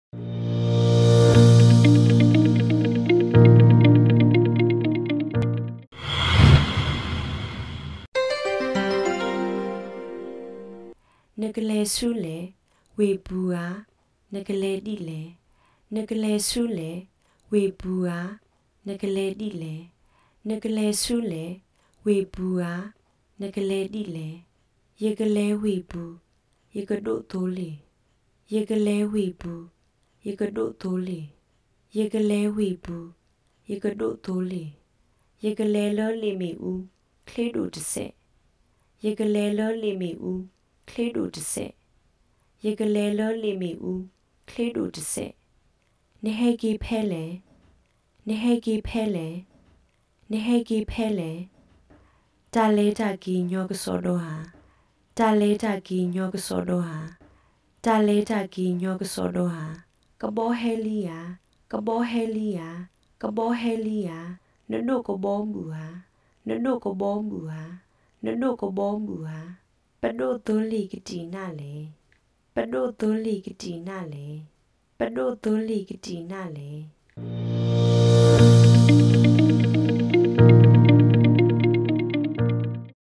Karen Daily Usages Episode 16
Welcome to Karen Center.Karen Daily Usages Episode 15 this video will teach you how to speak Karen Language. ကရင်ဘာသာ စကားဖြင့်မည်သို့ ပြောဆို လည်းကိုသင်ကြားပေးမှာဖြစ်ပါတယ် ။
Welcome to Karen Center .Karen Daily Usages Episode 16 this video will teach you how to speak Karen Language.